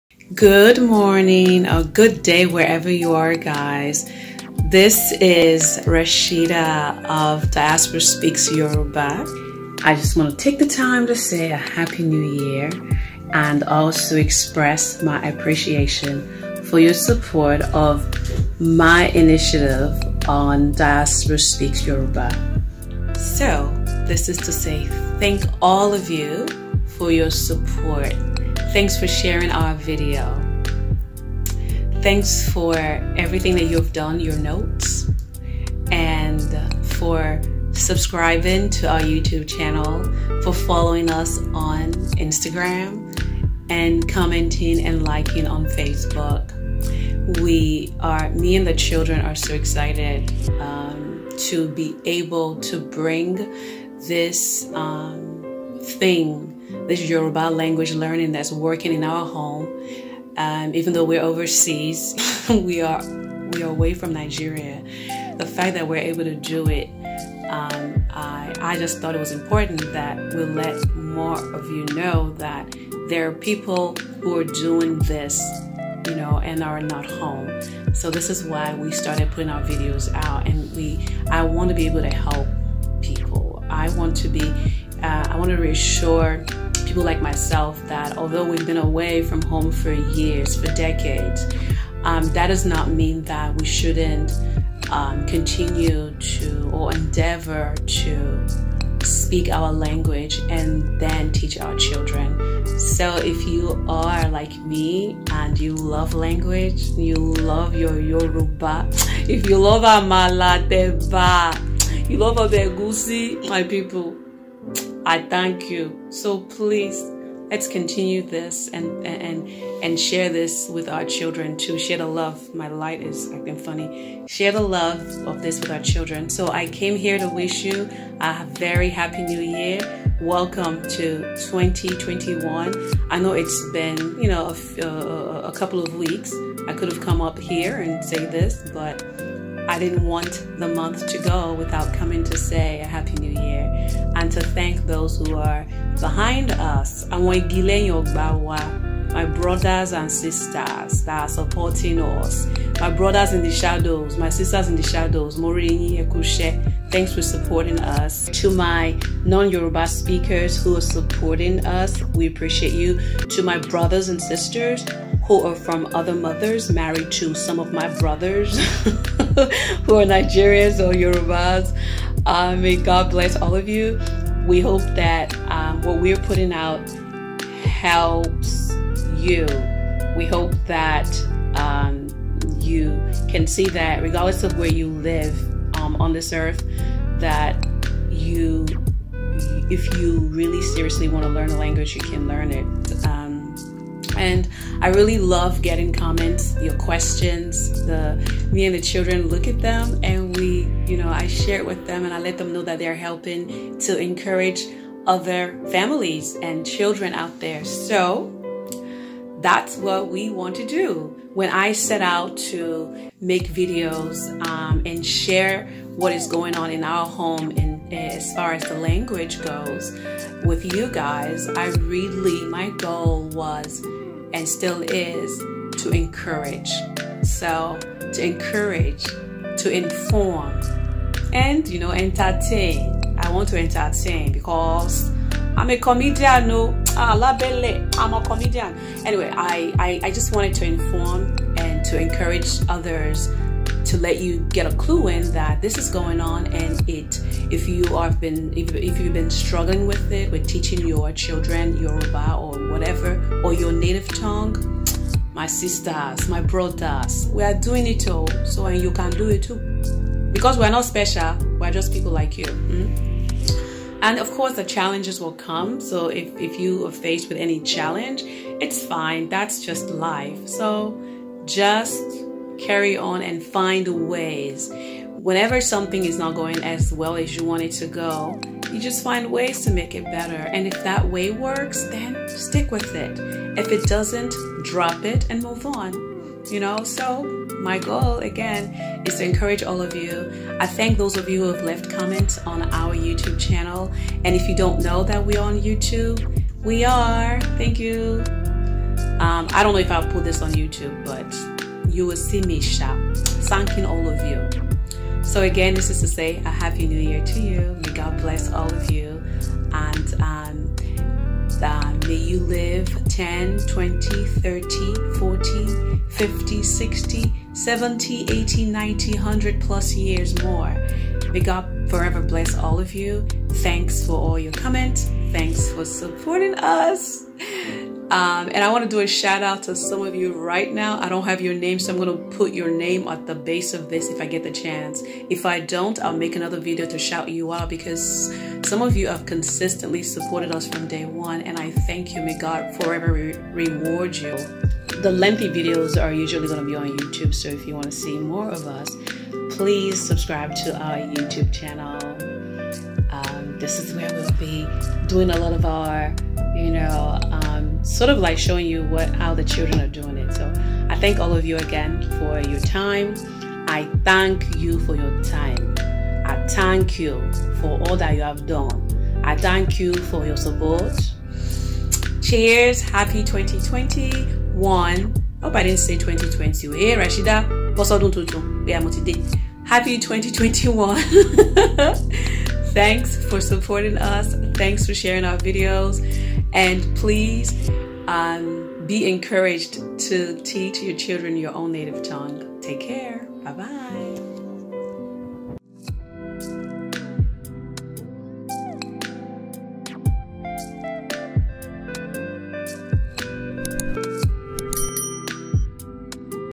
Diaspora Speaks Yoruba: Happy New Year!!!! & Some Encouraging Words
Happy New Year!!! This video is about me expressing our gratefulness for your support, and encouraging you to continue speaking Yoruba, and teaching your children.

Video Producer/Talent: Rashida Jones

We appreciate all the support you have shown, and hope to continue on our journey to raising culturally and identity-aware children.